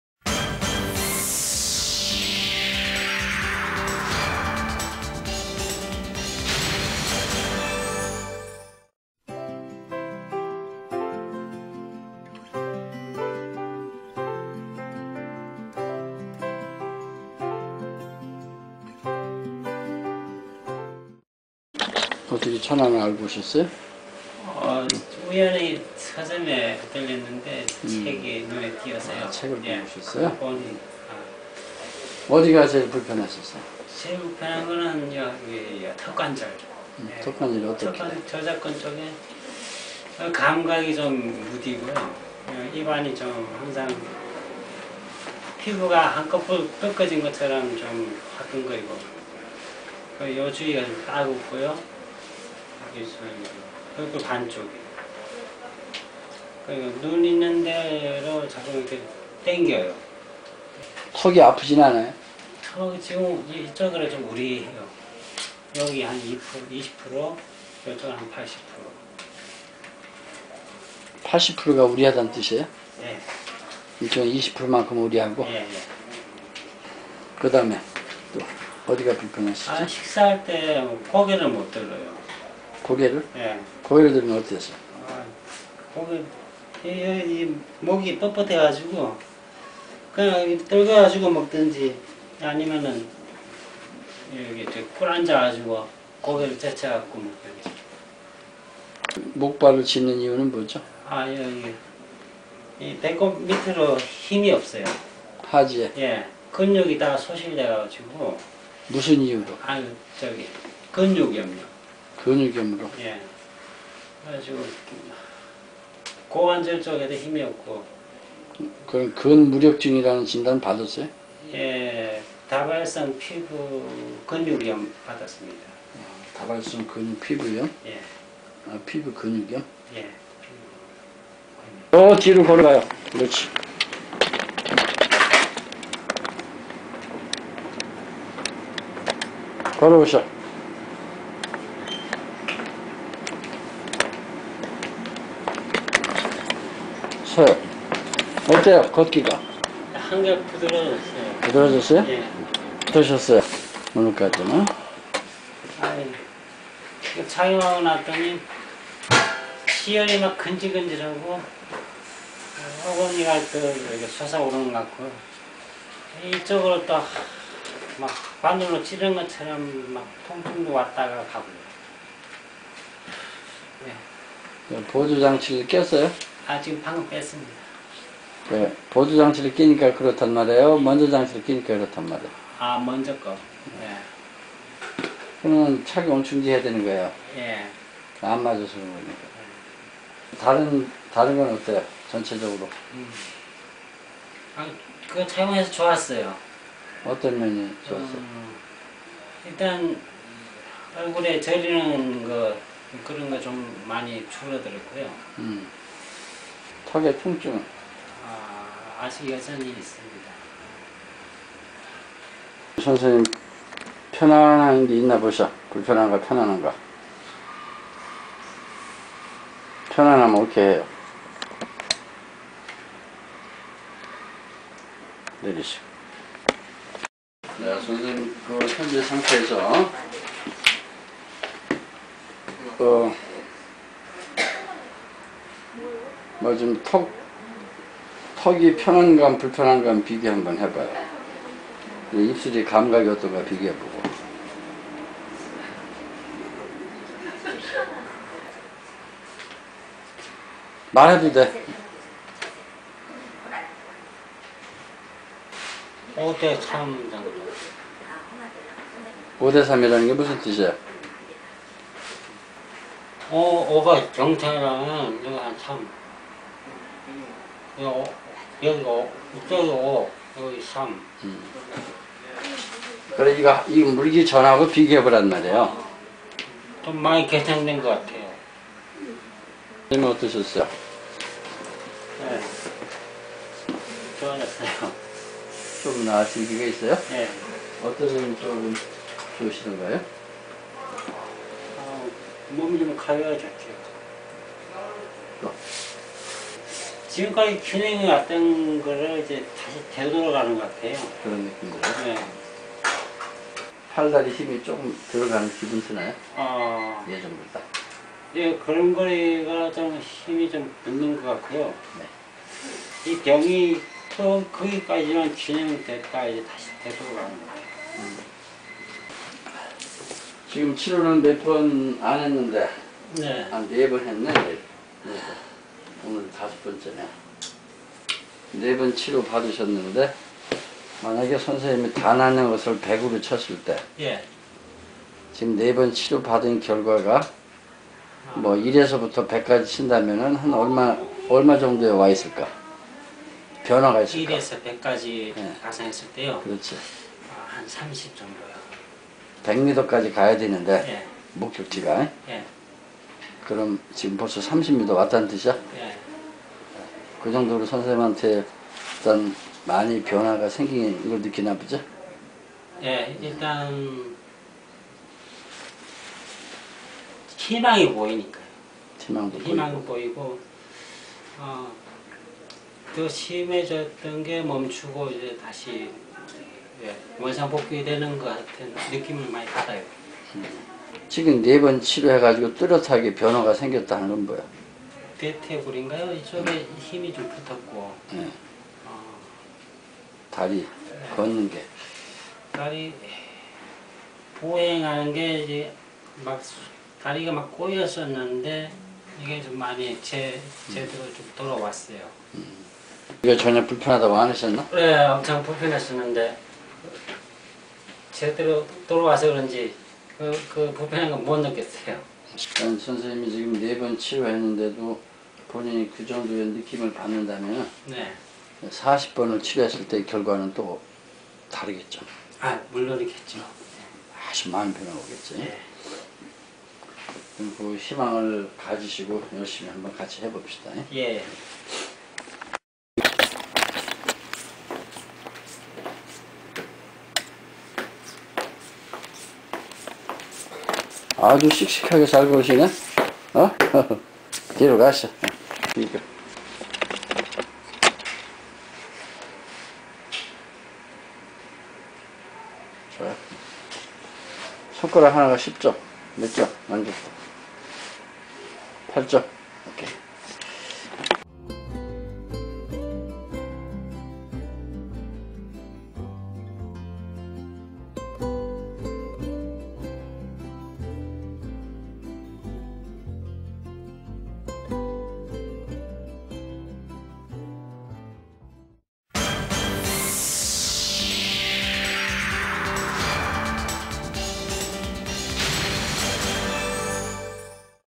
피부 근육염으로 인한 보행장애와 턱관절 이상 FCST 치료사례
[주증상]
피부 근육염으로 인한 보행장애와 턱관절 이상

[부증상]
1. 뒷목
2. 어깨통증
3. 손감각의 둔함
4. 만성피로
5. 안면경련, 마비
6. 이 외 악관절이상 증후군 47가지